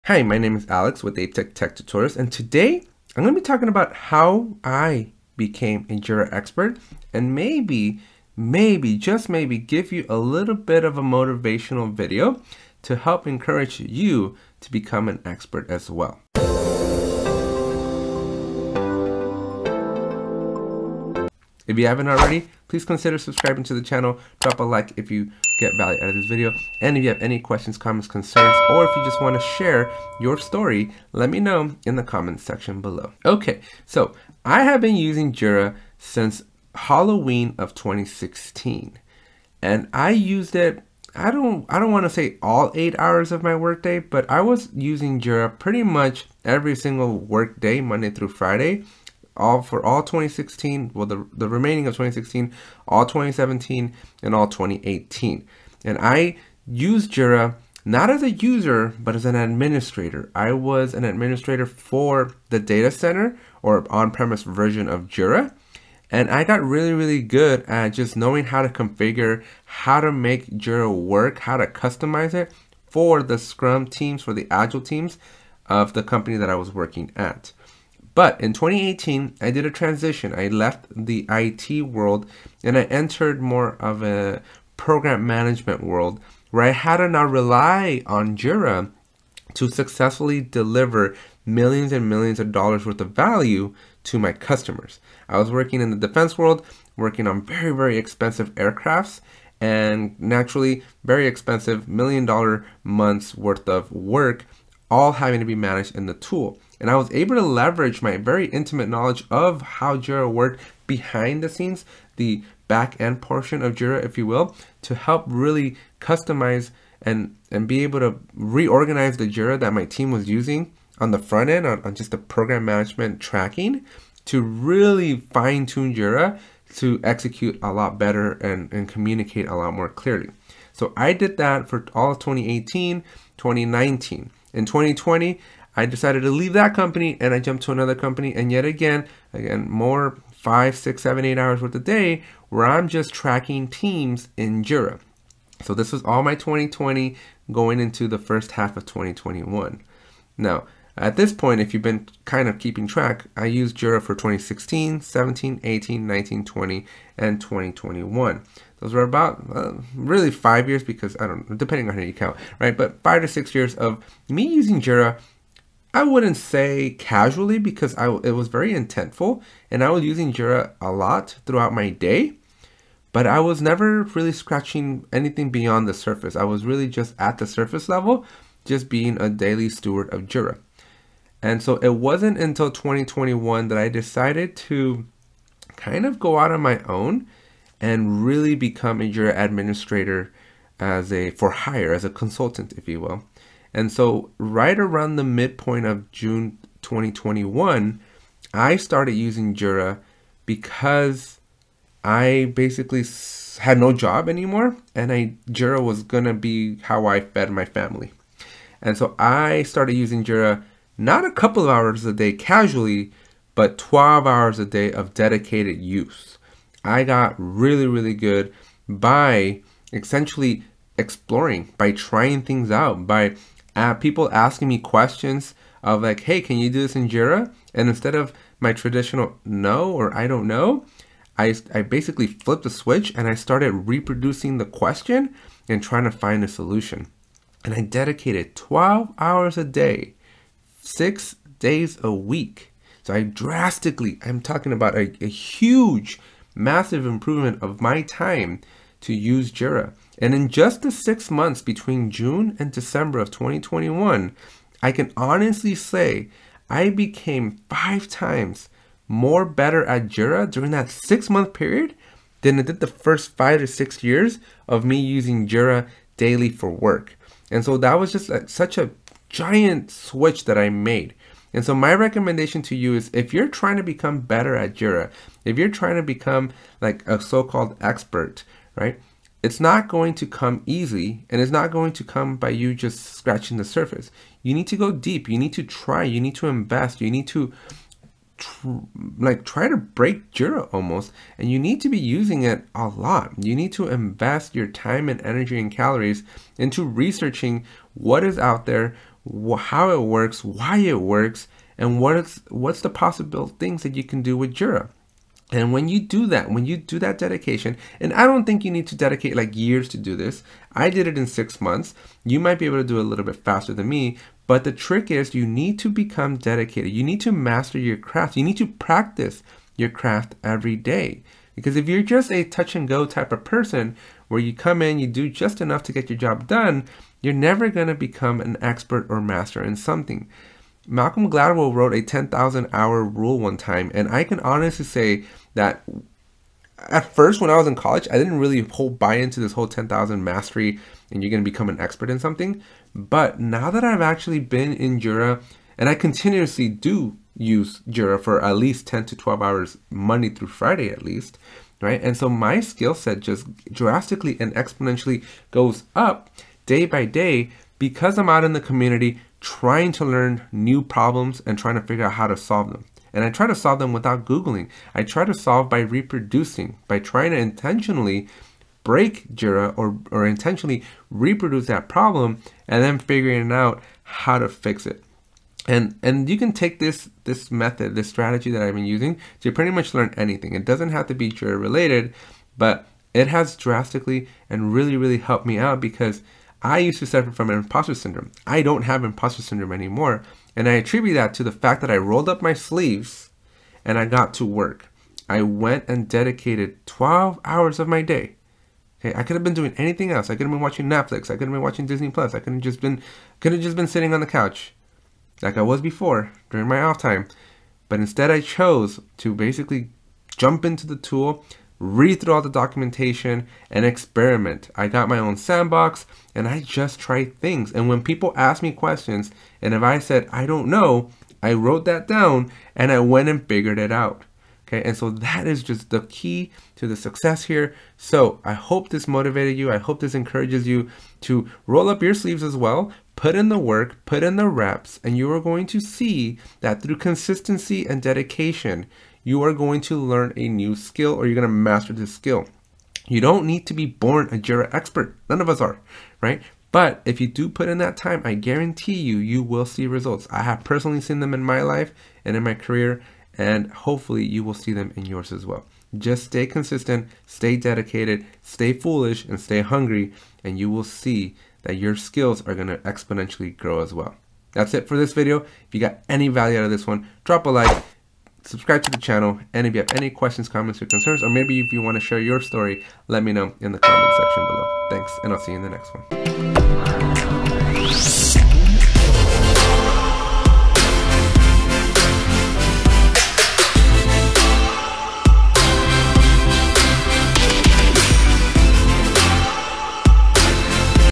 Become a Jira Expert by Making this Simple Change | Atlassian Jira
In this atlassian jira video, I am going to give you my best advice on how to become a Jira expert. I suffered with impostor syndrome for my entire professional career. About a year ago, I decided I would no longer let this fear control me. I decided to roll up my sleeves and started using Jira for 12 hours each and every day. Eventually, after only a short few months, I got really good at Jira. Every time I didn't know how to do something, rather than walking away, I jumped into Jira and figured it out. I made this video to motivate future Jira practitioners and just about anyone wanting to learn a new skill.